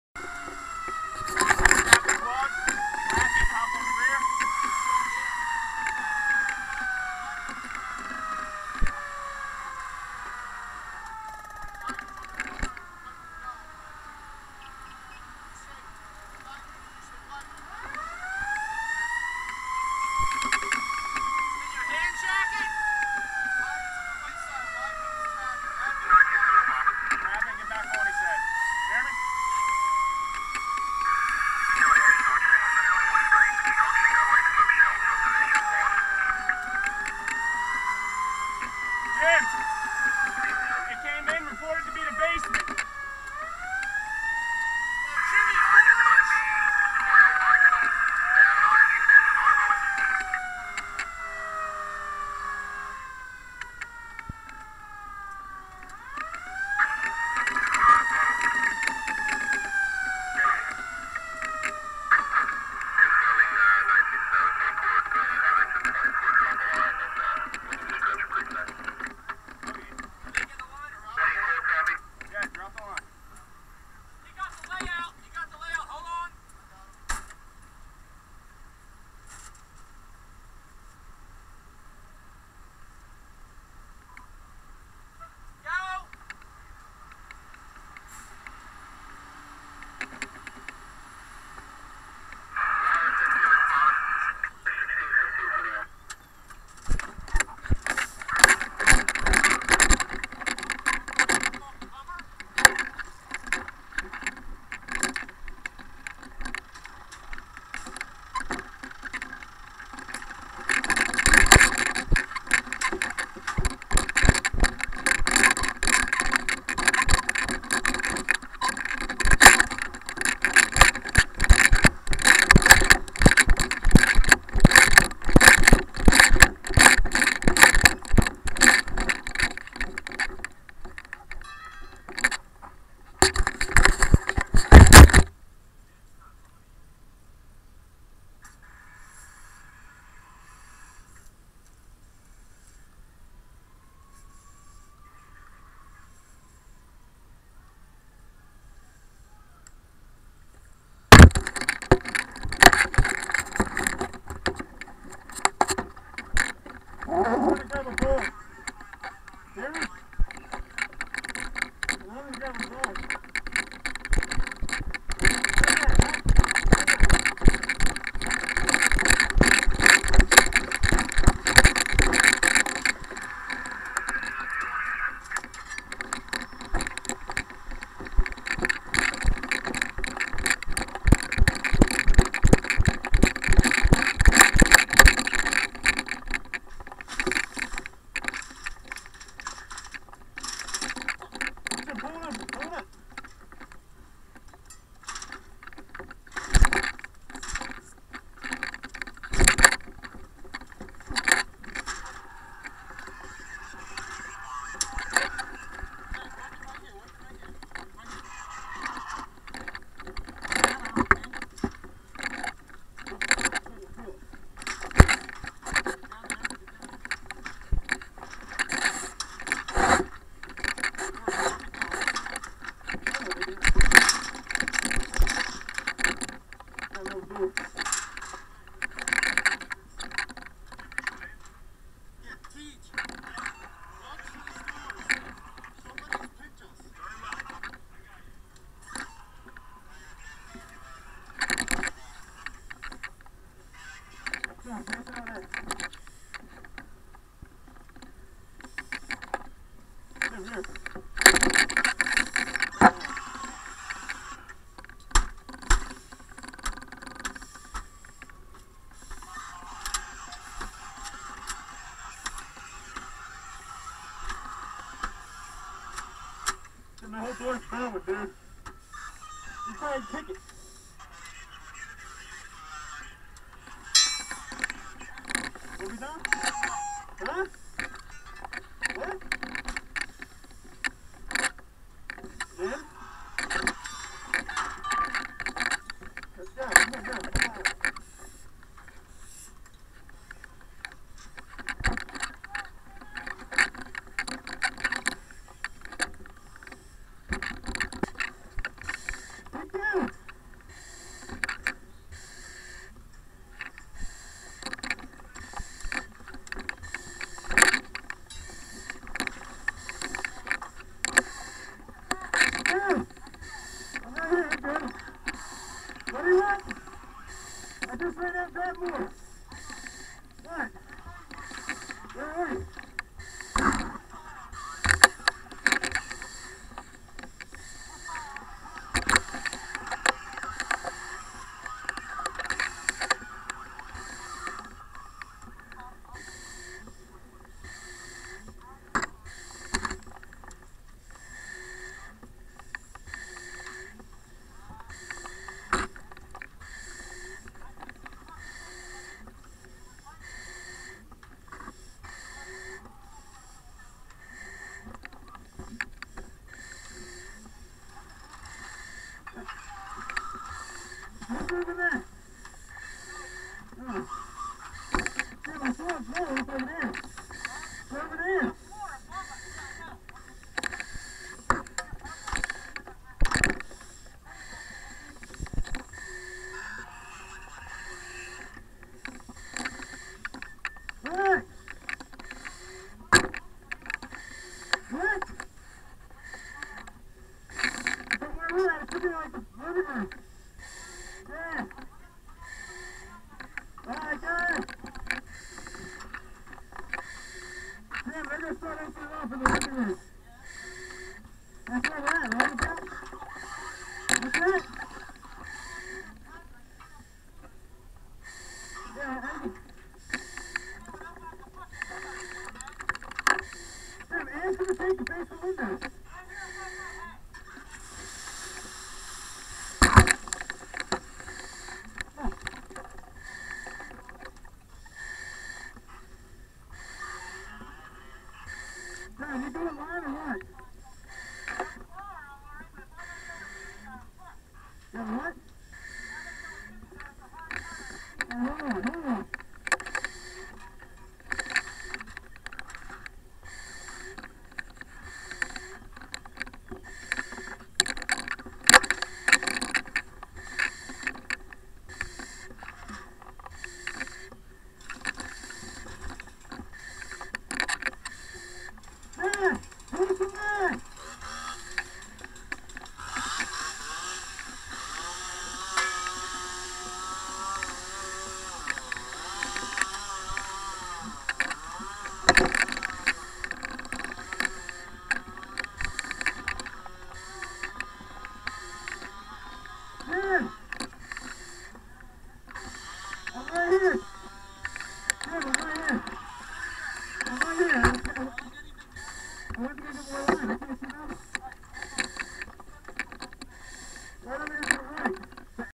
Basement Fire
On 6-29-13, Engine 24 (East Lansdowne) was dispatched to the unit block of Stratford Ave (Lansdowne) for a house fire. Upon arrival, Chief 24 advised all units of a working basement fire, in a 3 story, 20x50' balloon frame twin. Engine 24 dropped the 3" and stretched the 150' bumper line to the Delta side "Bilco" doors. Engine 24's officer and back up FF, forced entry and found thick smoke conditions throughout the basement. While the Engine crew advanced through the basement, a second line was stretched to protect the 1st floor. With minimal ventilation available, and zero visibility, The Engine crew did finally locate the fire by the sound of the fire. The fire involving 2 rooms was accessed and quickly extinguished and crews remained on scene for a total of 2 hours. (Sorry, no visible fire can be seen in this video, due to our other helmet cameras still being OOS)